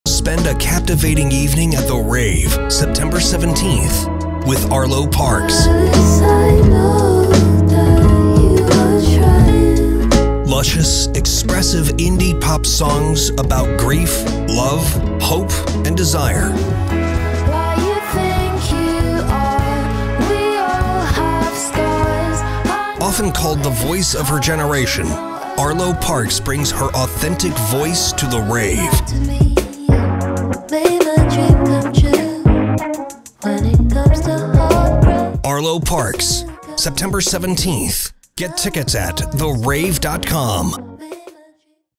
Arlo Parks - September 17, 2024
ARLO PARKS
Tuesday, September 17, 2024 at 8pm
(doors scheduled to open at 7PM)

The Rave/Eagles Club
All Ages

Advance tickets start at $28.00. Day Of Show tickets may be higher.

All information subject to change. for any changes that may have been made since this listing was created.

In Arlo Parks’ world, words are as useful as photographs. Luscious, expressive vignettes pepper the poetic lyrics in her sweet, ruminative indie pop songs. She’s inspired by an eclectic mix of artists from Radiohead to Portishead, and Sufjan Stevens to Solange.